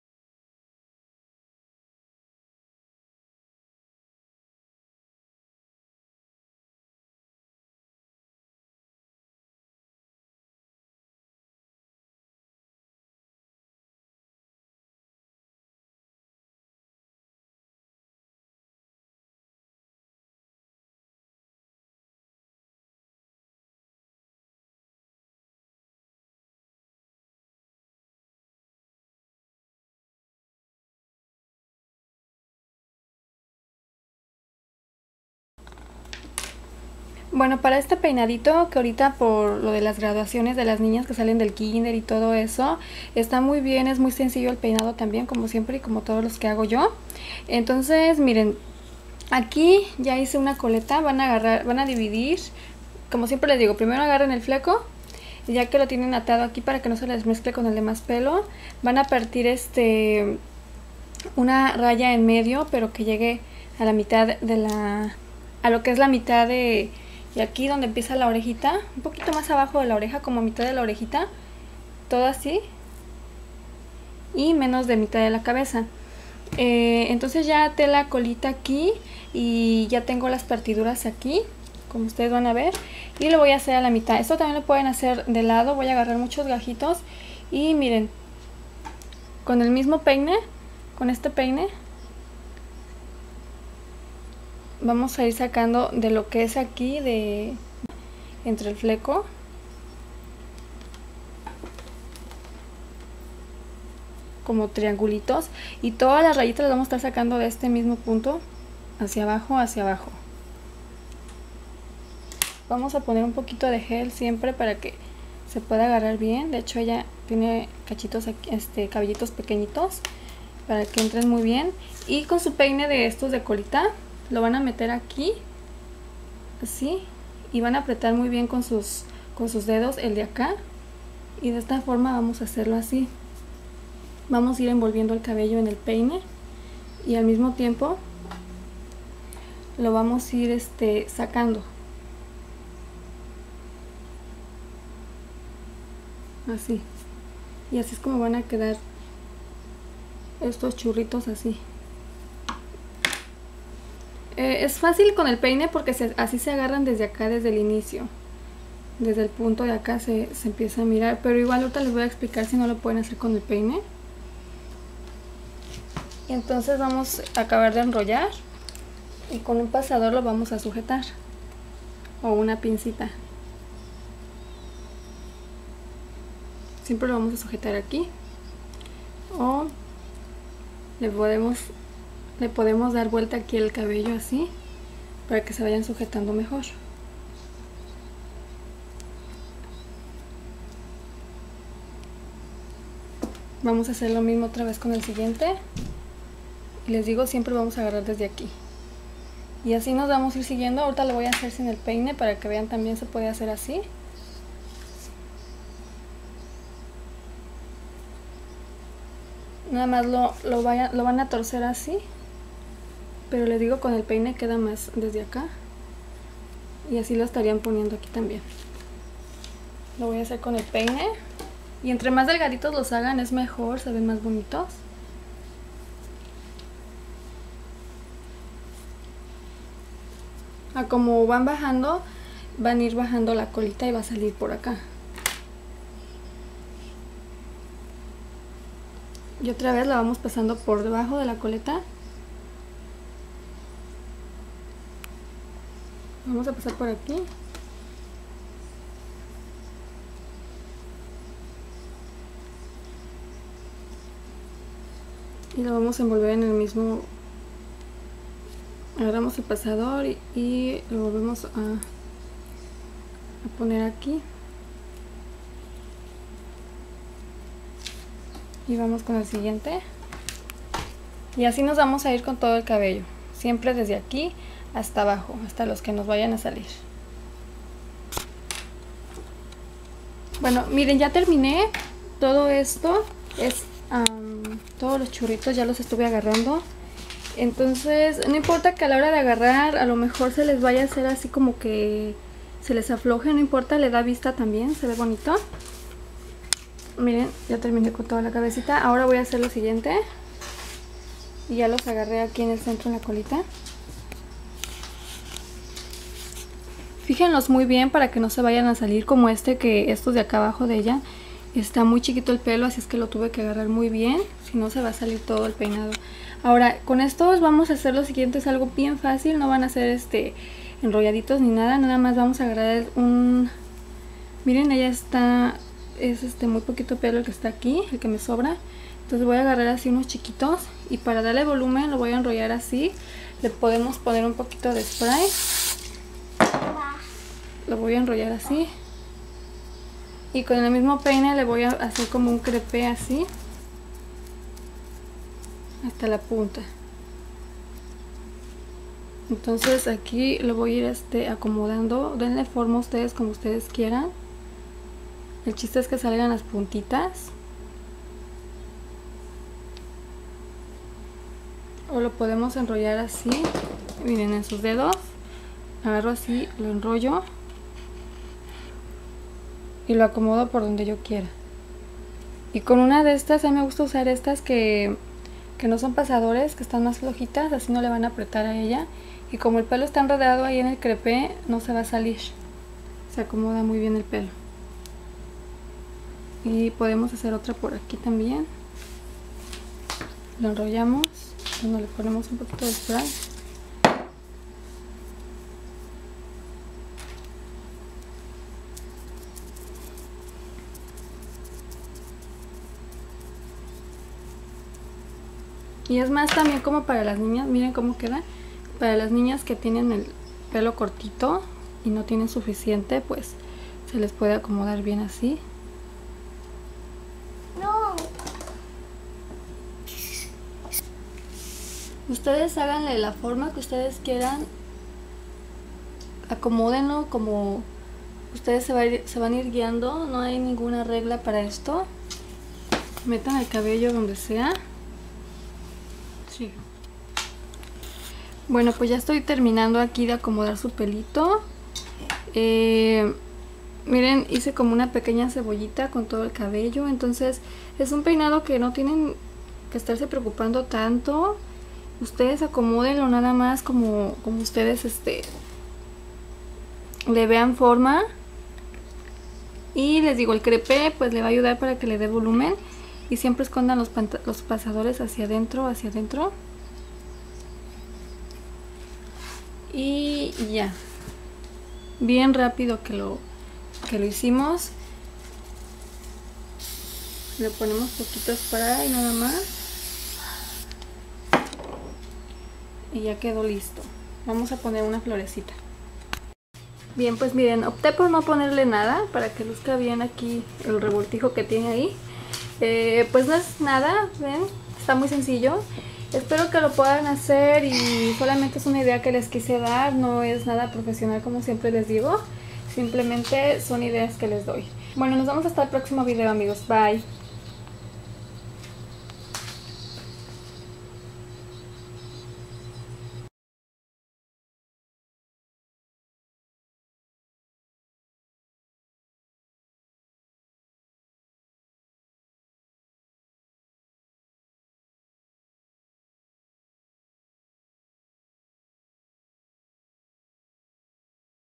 PEINADOS SENCILLOS Y FACIL PARA NINAS - OPCION PARA GRADUACION aleliamada.
PEINADOS SENCILLOS, OPCION PARA NINAS DE CABELLO CORTO.
PARA SEGUIRME POR FACEBOOK..